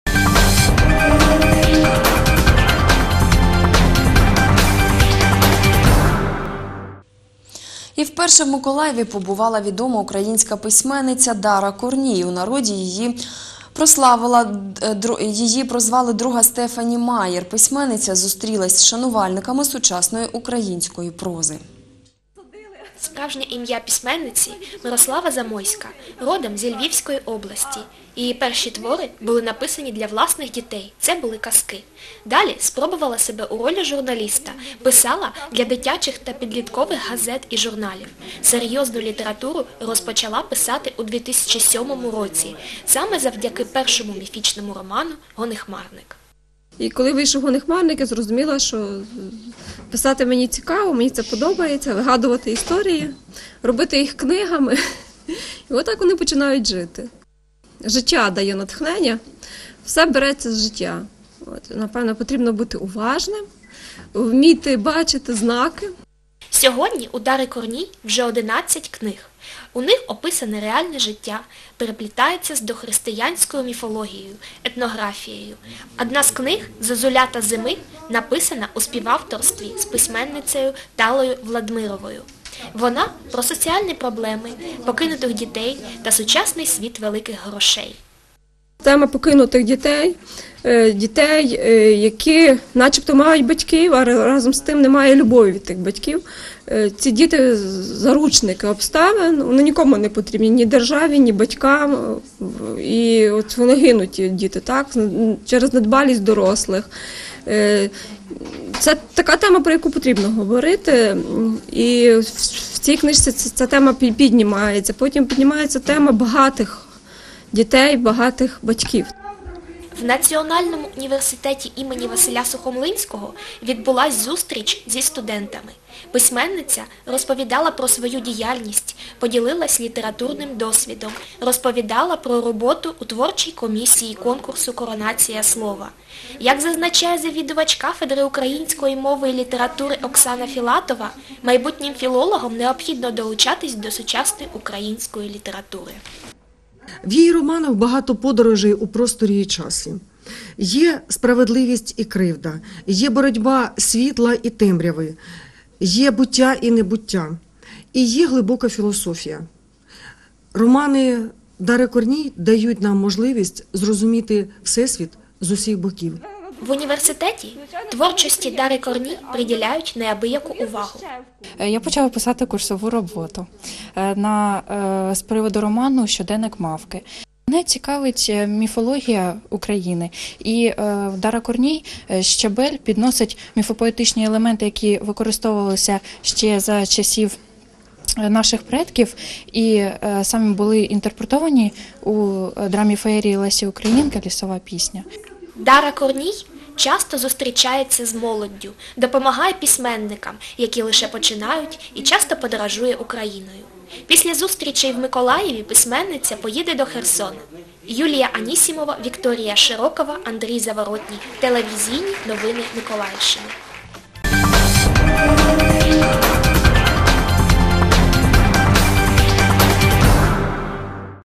Приїзд Дари Корній викликав чималий ажіотаж
Дара Корній у Миколаєві: українська письменниця зустрілась  з шанувальниками сучасної української прози.

Автори сюжету: Юлія Анісімова, Вікторія Широкова.